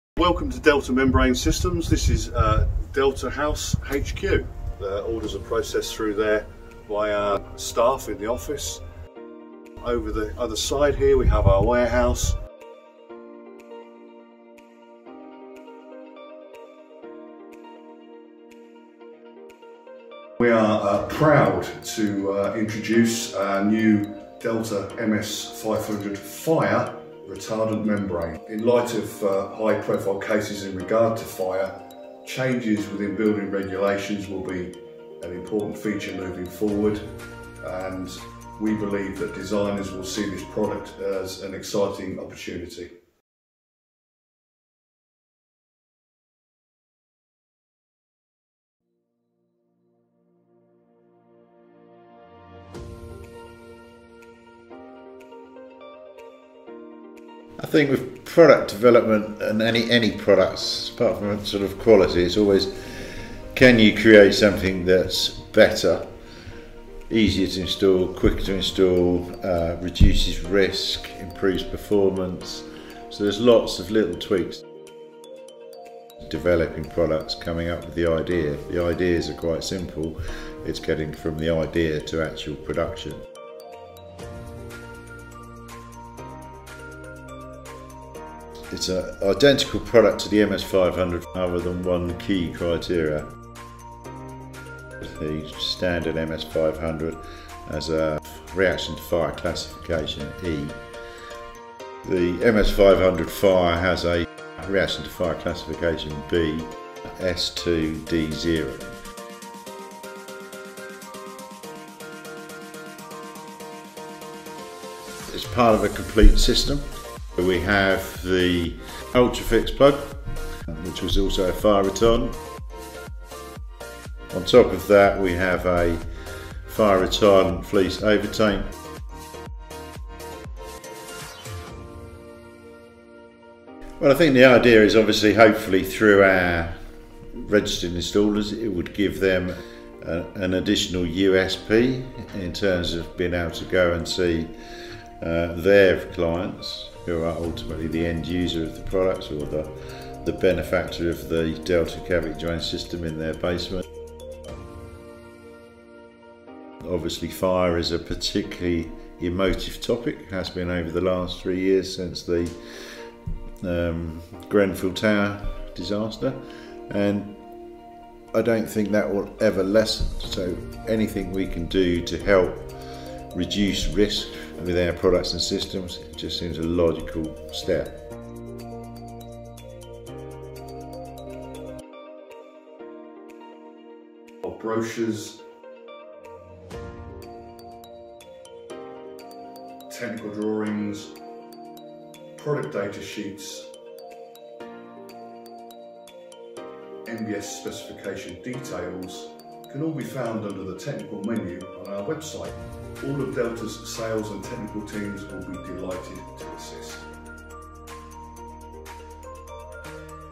Delta MS 500 Fire Retardant, Type C Structural Waterproofing Launch
Innovation is a pulse at Delta, we thrive on making the impossible, possible.

As with all Delta products, no one knows protection like we know protection. We’ve learnt how to protect buildings from the most extreme conditions imaginable all over the world. Quality is consistent because of our  unparalleled manufacturing and quality control processes.

Delta’s new innovative fire retardant MS 500 membrane gives a unique opportunity to the structural waterproofing sector for Type C systems to be used in reducing the spread of fire or where installation has not previously been possible for fire safety reasons.

Delta MS 500 Fire offers a B-S2, d0 Euroclass fire rating (EN 13501-1:2018) whilst maintaining its strength, durability, functionality and workability.

Delta’s new fire retardant Type C Cavity Drainage System provides architectural freedom and allows systems to be used in not only deep basements but where national Building Regulations Euroclass B or C are  required, meeting building requirements of today, but also that of tomorrow.